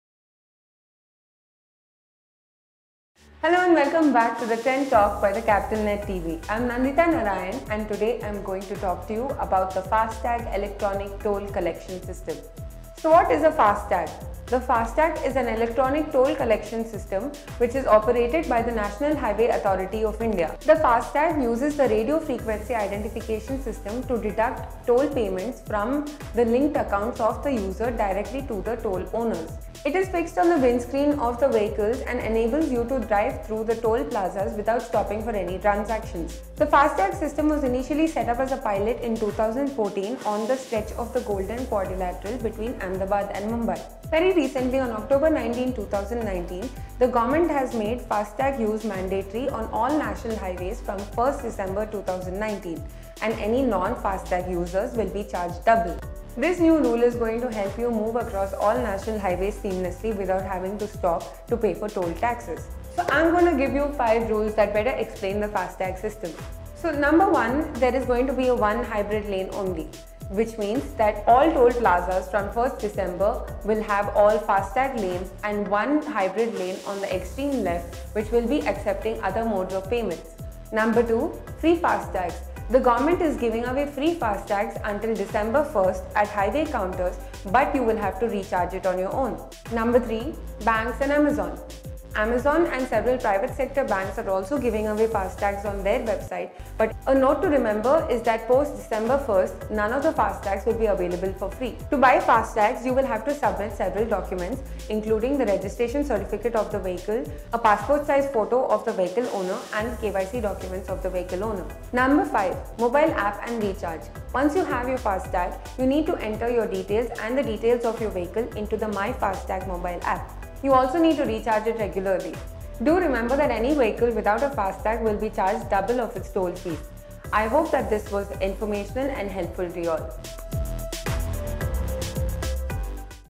Trend Talk by TheCapitalNet TV - What is a FASTag?
Trend Talk by TheCapitalNet TV is an initiative which talks about trending topics both in the global and national context.

What is a FASTag? This episode talks about the mandatory use of ‘FASTags’ for online payment of toll taxes.